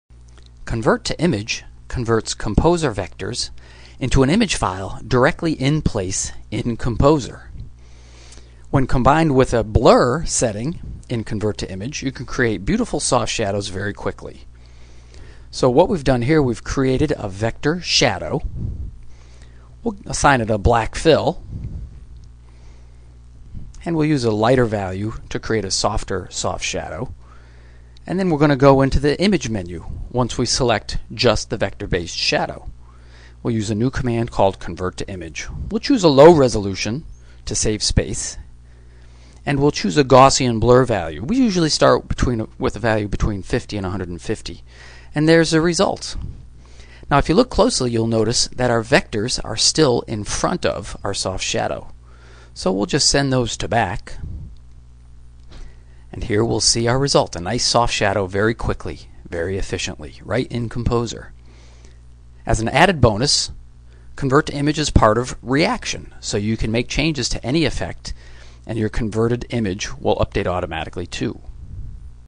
Convert to Image
Create shadows, then convert to soft shadows right in Composer. Convert any Composer vectors into an image to apply new image effects or to apply the STC Photo halftone.